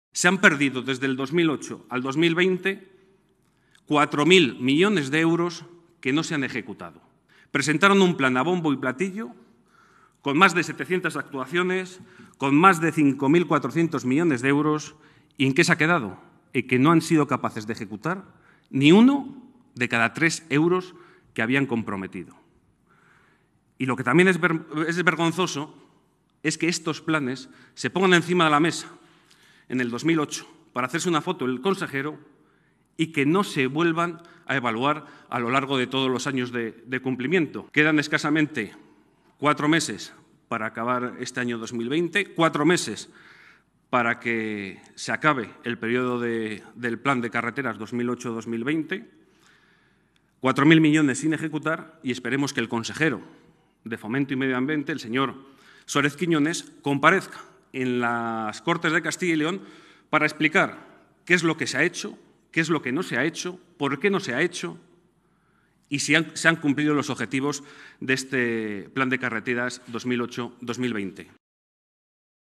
13/08/2020 Ángel Hernández comparece ante la prensa - Plan de carreteras de la Comunidad
La Junta tiene que explicar por qué no ha ejecutado ni tres euros de cada diez del Plan de Carreteras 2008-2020.

❌ Quedan 4 meses para que finalice el plan y todavía restan por invertir más de 4.000 millones de euros.